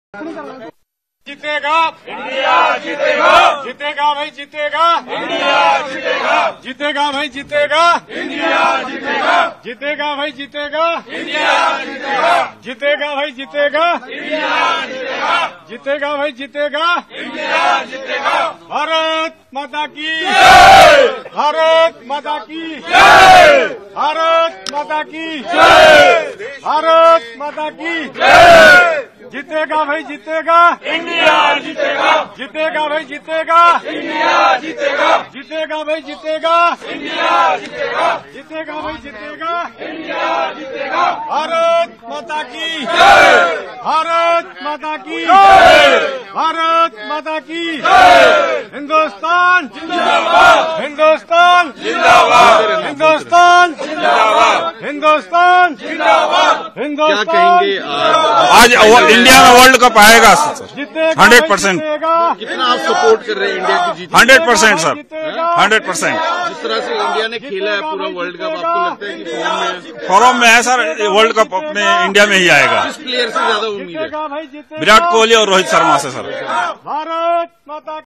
ଭାରତୀୟ ଦଳକୁ CRPF ଯବାନଙ୍କ ଶୁଭେଚ୍ଛା ବାର୍ତ୍ତା || World Cup Final || India vs Australia
ଭାରତୀୟ ଦଳକୁ CRPF ଯବାନଙ୍କ ଶୁଭେଚ୍ଛା ବାର୍ତ୍ତା...।